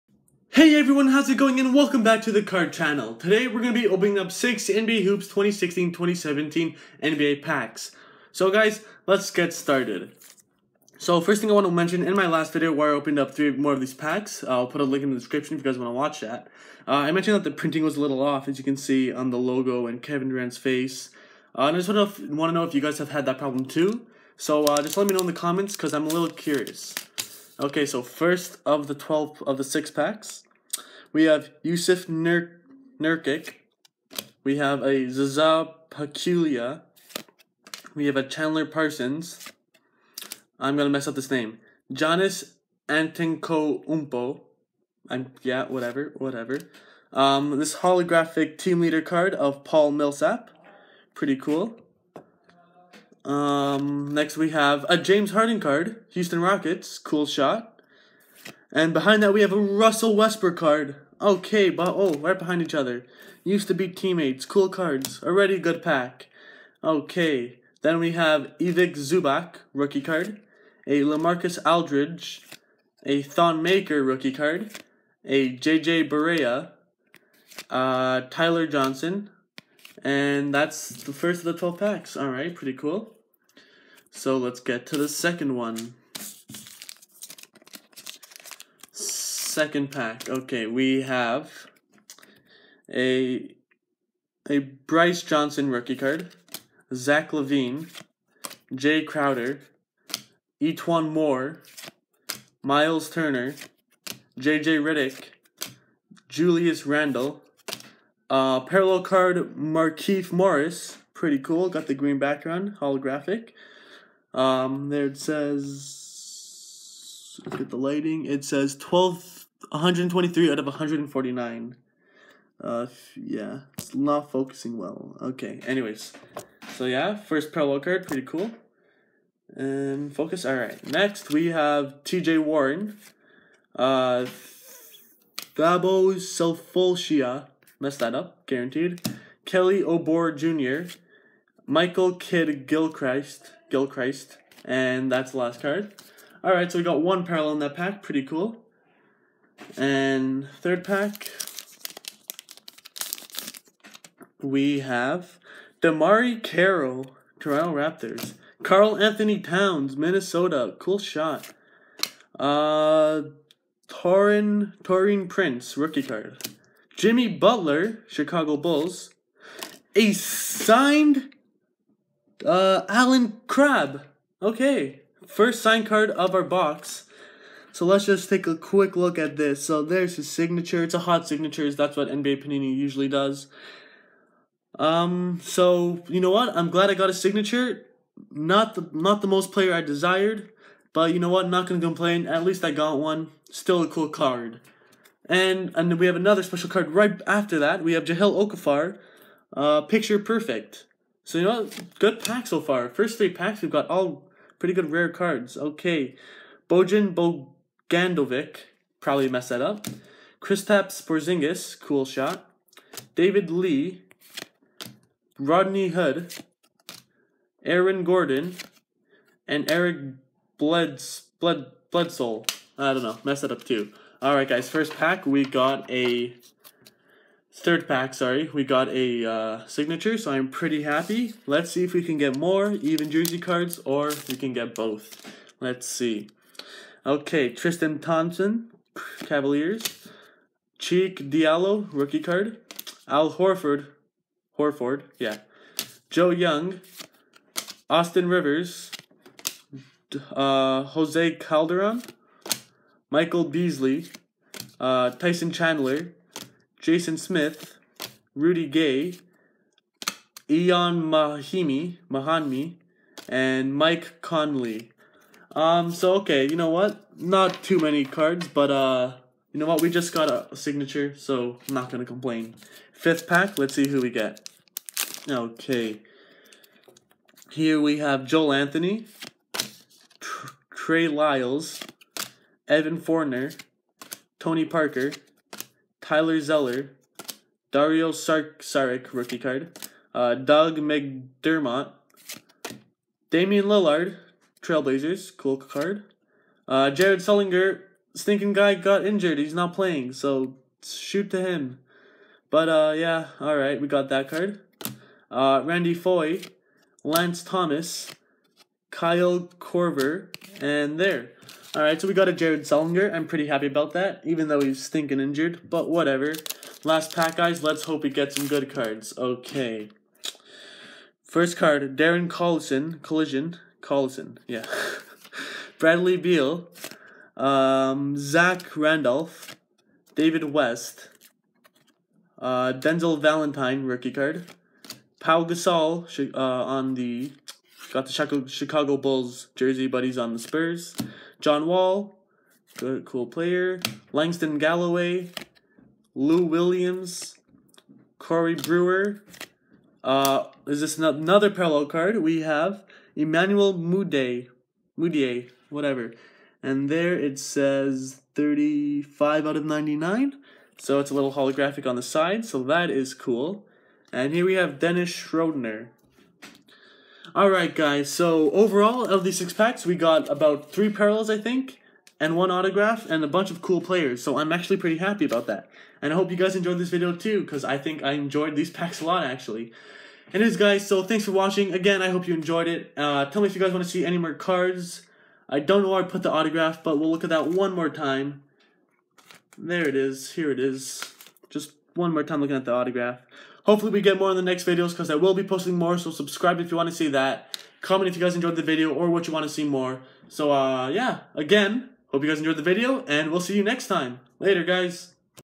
OPENING 6 PANINI NBA HOOPS 2016-2017 PACKS
Today we open 6 NBA HOOPS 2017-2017 basketball packs with a special card. Enjoy!